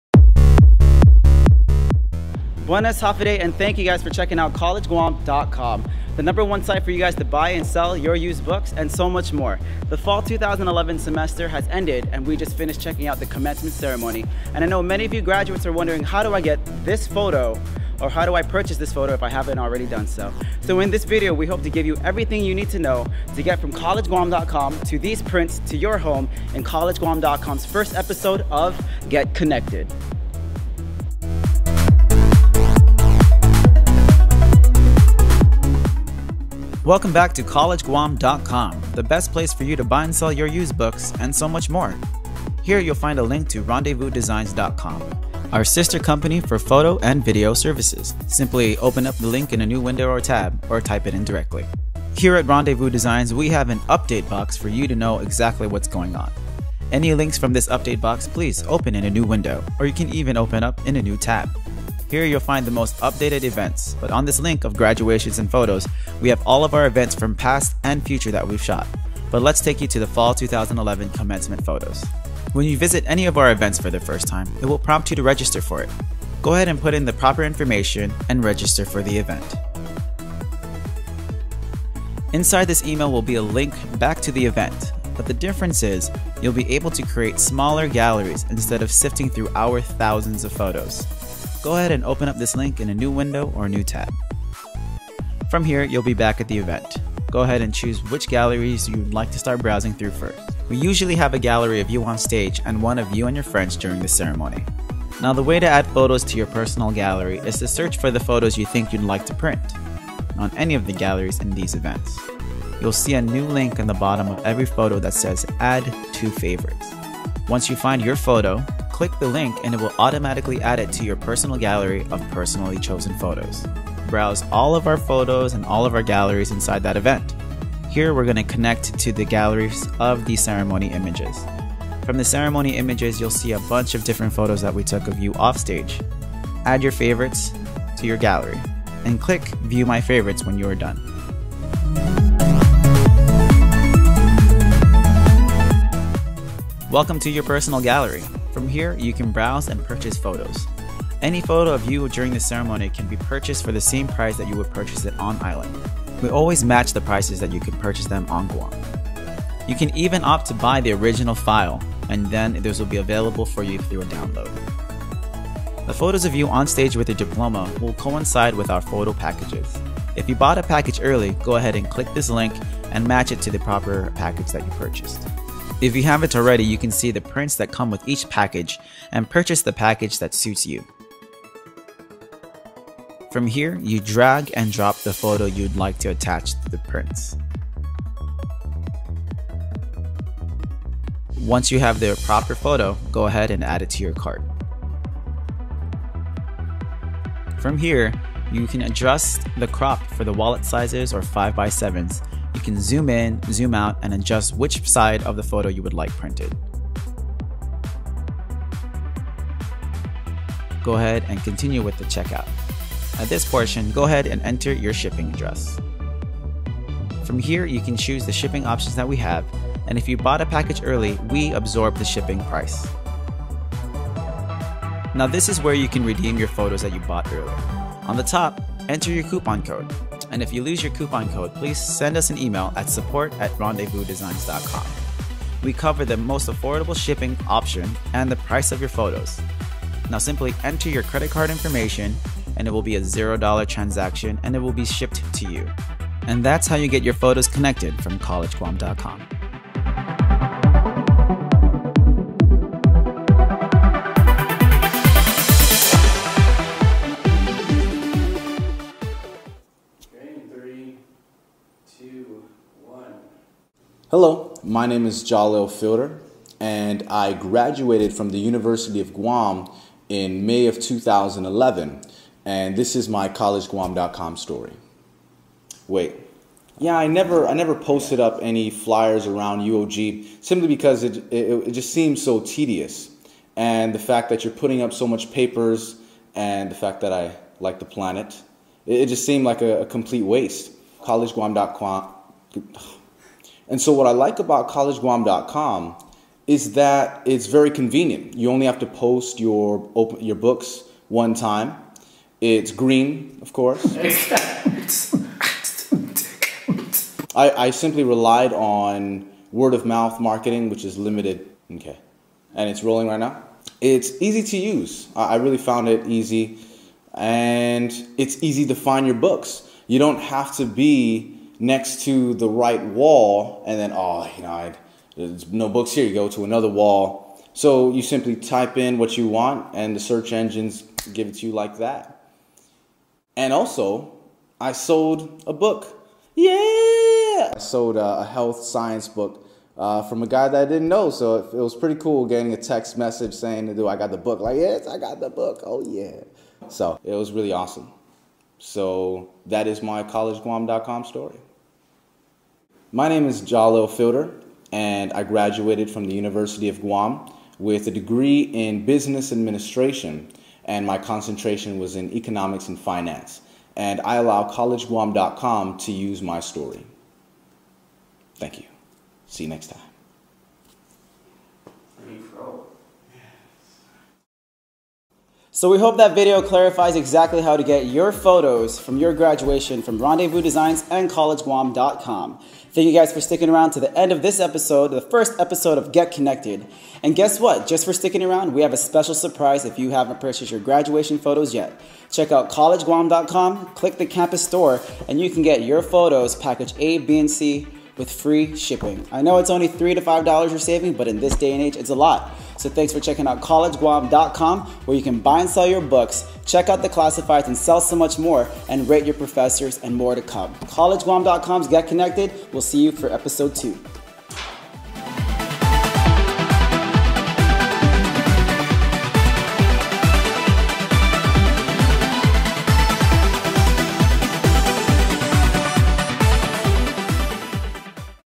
Get Connected 1 - Rendezvous Designs & Graduation Photos
In this video, find out how to redeem your purchased graduation photos from Rendezvous Designs and CollegeGuam.com. Stick around until the end of the video for great incentives.